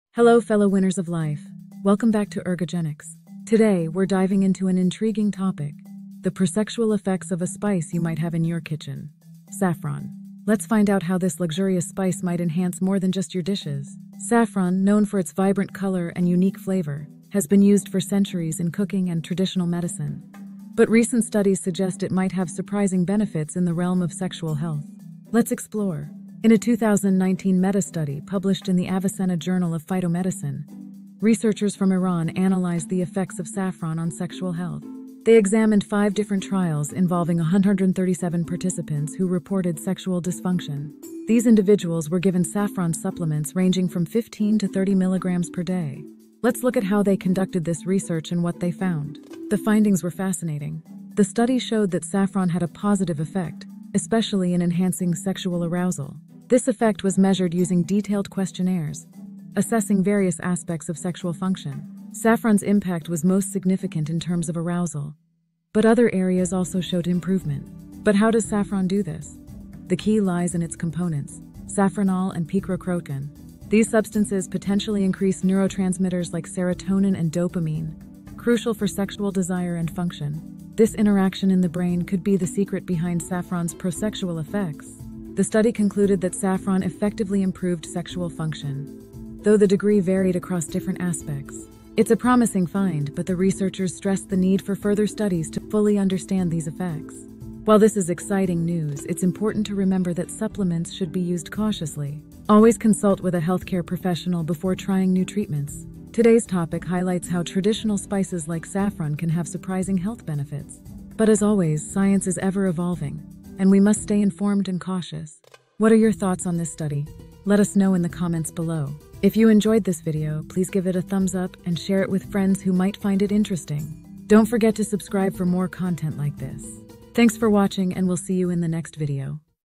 Libido Enhancement | Saffron
Checkout my instagram! 🌟 In this eye-opening video, we delve into the luxurious world of saffron and its unexpected benefits in the realm of intimacy and sexual health. 🌺 Uncover the secrets of this ancient spice and how it could potentially transform your romantic life.

🔍 Our deep dive into the 2019 meta-study from the Avicenna Journal of Phytomedicine reveals how saffron supplements might enhance sexual arousal and overall well-being. 📊

👉 Don't miss out on these intriguing insights that could add some extra spice to your life!